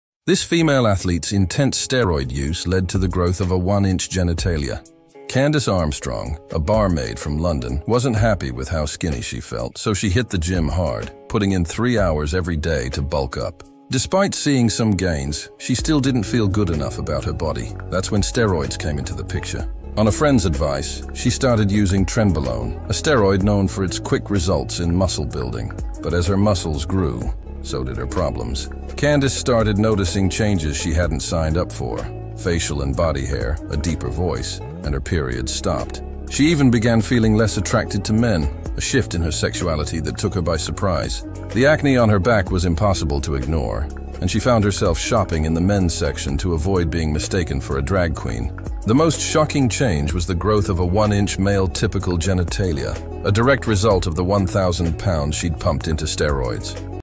She Grew a One-inch Genitalia from Using Steroids
Explore the gripping journey of a determined female bodybuilder whose pursuit of the perfect physique through steroids led to unexpected and life-altering changes. This eye-opening story delves into the high stakes of performance enhancement, revealing the profound physical and psychological impacts that challenge the boundaries of human transformation.